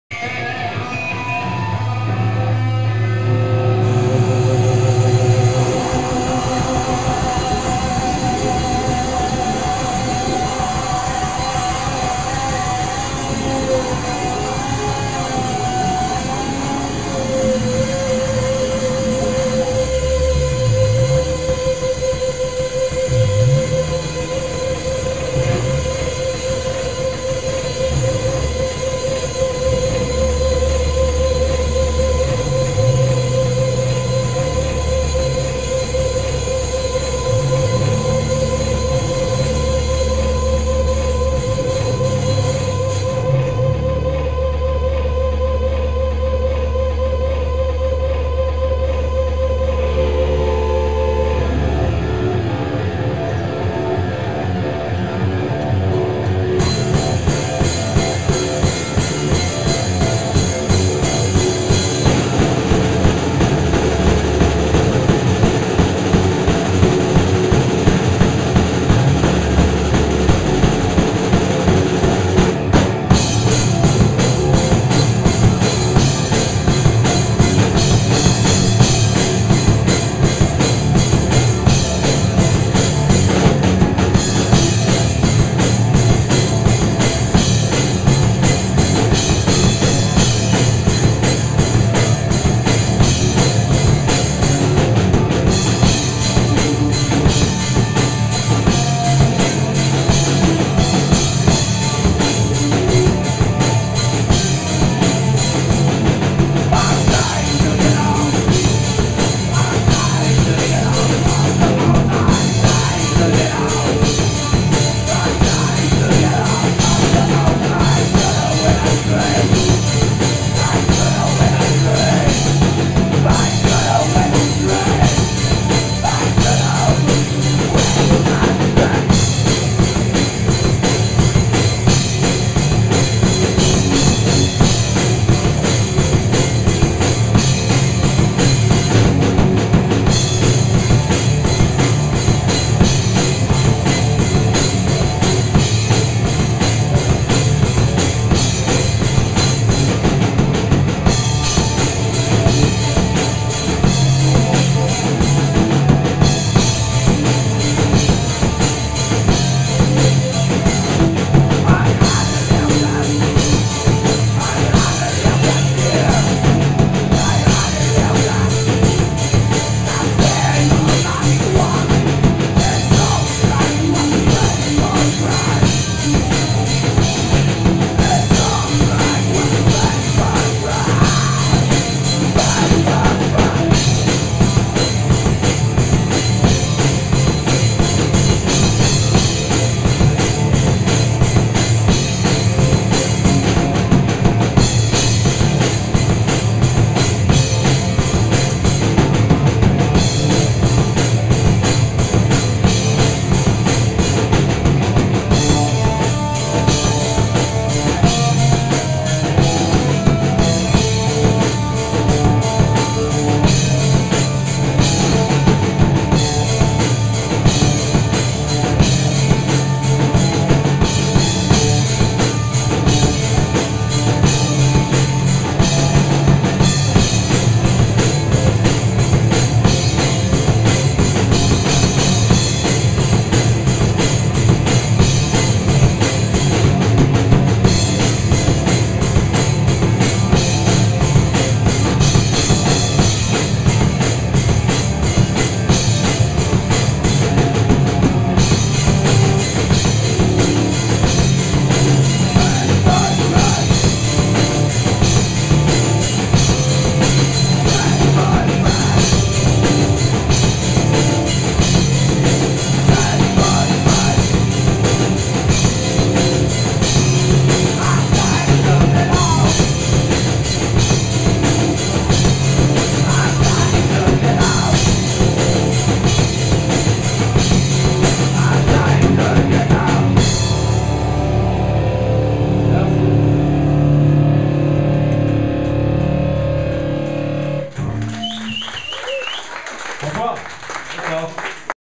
MISSILES OF OCTOBER live in Brussels on 22.10.2012 p.1
Lionel Beyet: bass & scream
Bob Seytor: drums
Mathias Salas: guitar & scream